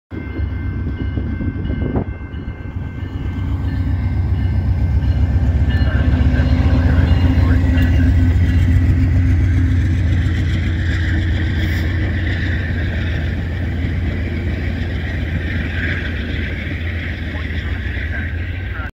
(Detour) (39 Minutes Late!) VIA 73 - 903 With Budd Cars In Burlington Ontario Halton Subdivision
As Track Work takes place along the Lakeshore West Line between Toronto Union Station & Burlington GO Station, VIA Rail Canada Trains are Detouring along the Halton Subdivision and GO Transit Trains are operating every 4 Hours between Aldershot & Niagara Falls